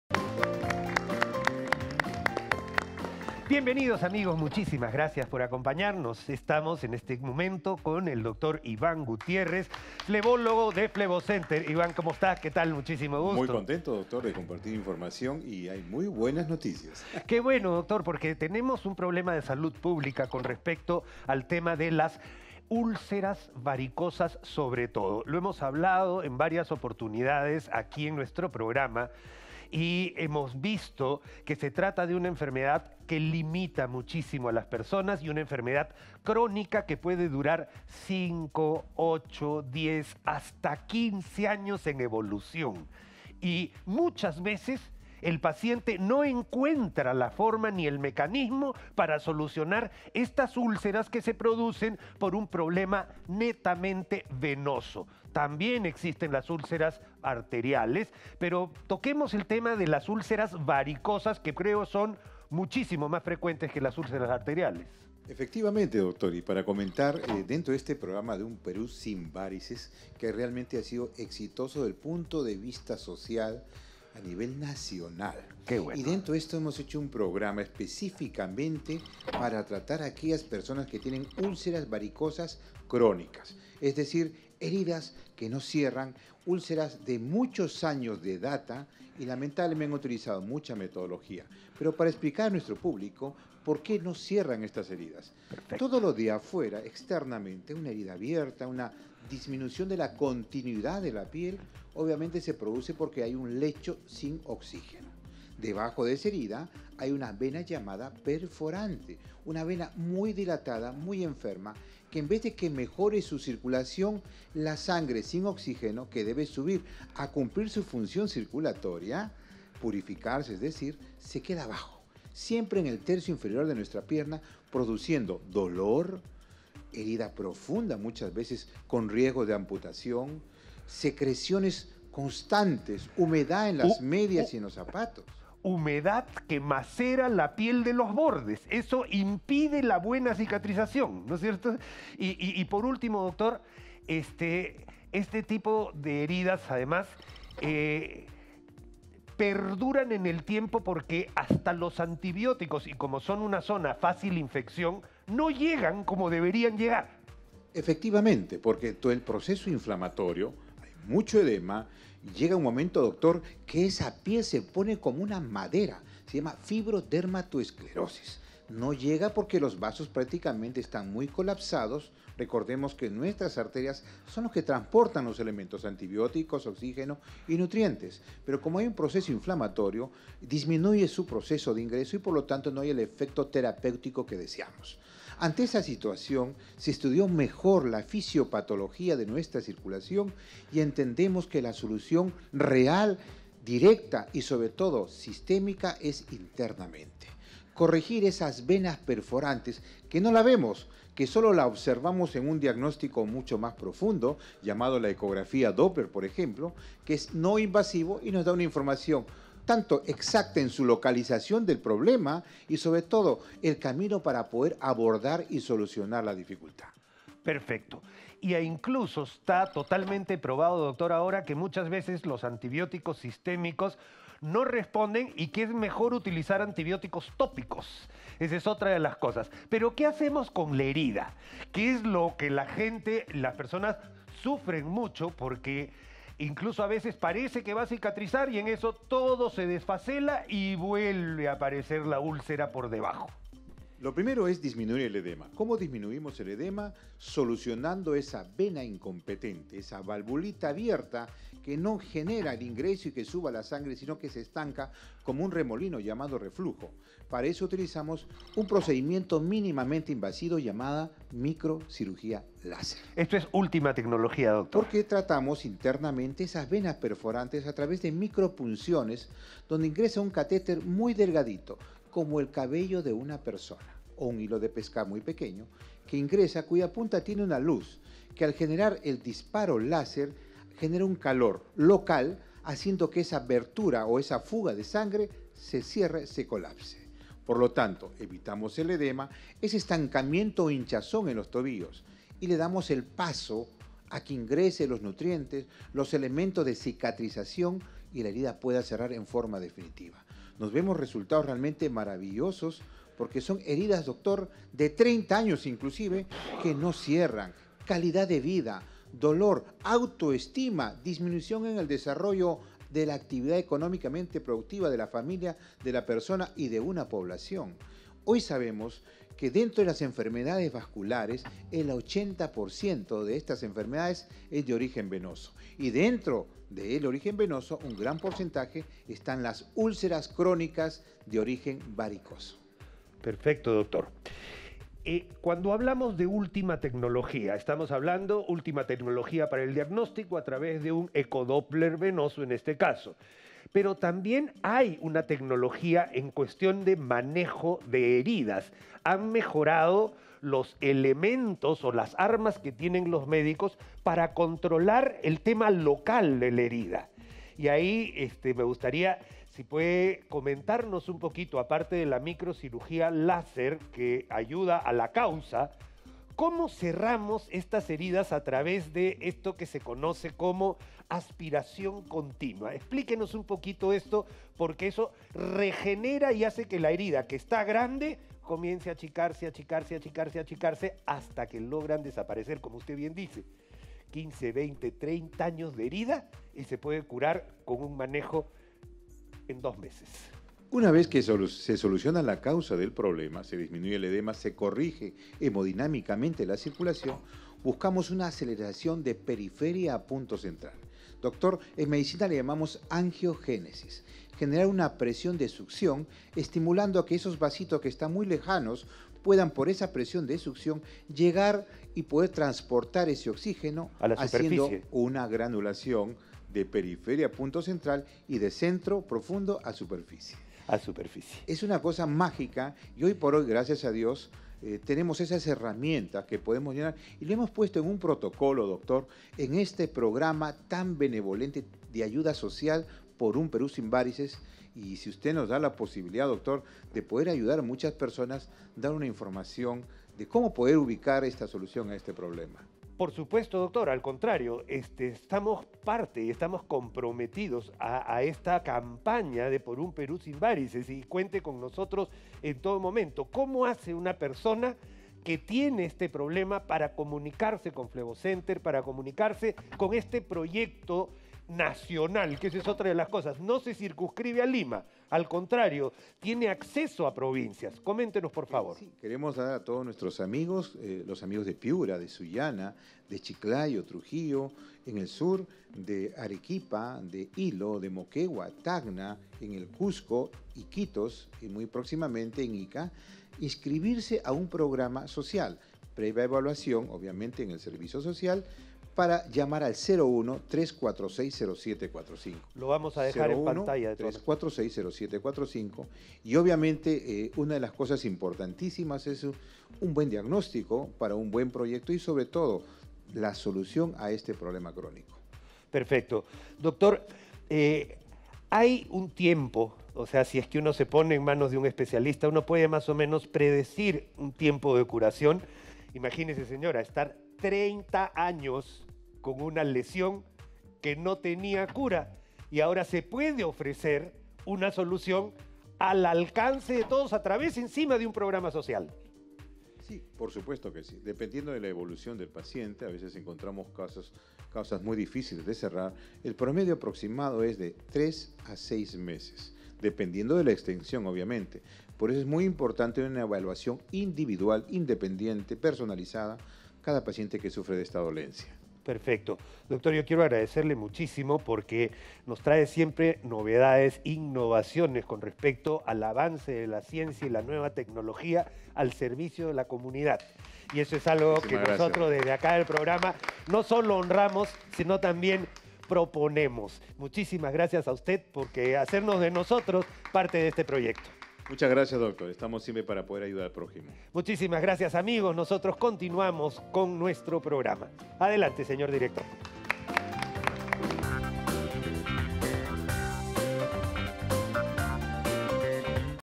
Dr. Borda: ¿Cuál es el tratamiento de las úlceras varicosas? (Programa Willax 27/07/21)
Las úlceras varicosas son una enfermedad crónica que puede durar hasta 15 años en evolución. El Dr. Iván Gutiérrez nos cuenta más acerca de esta afección, su relación con las várices y cómo podemos curarla.

Recuerda, si padeces de várices no dudes en consultar a un médico flebólogo para un correcto diagnóstico y tratamiento.